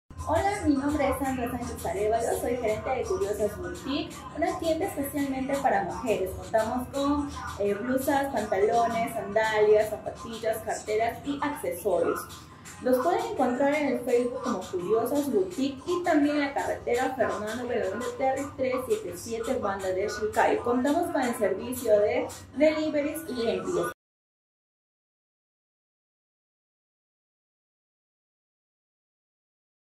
Somos Chamba - Sandra Sánchez Arévalo - Curiosas Boutique
Sandra Sánchez es gerente de “Curiosas Boutique”.  Es una tienda especializada en prendas de vestir para damas. Dentro de su oferta puedes encontrar blusas, tops en diferentes modelos y colores, pantalones, sandalias, zapatillas, carteras y accesorios. Puedes solicitar tus pedidos desde la comodidad de tu hogar ya que realizan el servicio de delivery.

Carretera Fernando Belaúnde Terri 377 Banda de Shilcayo
 - Tarapoto, San Martín.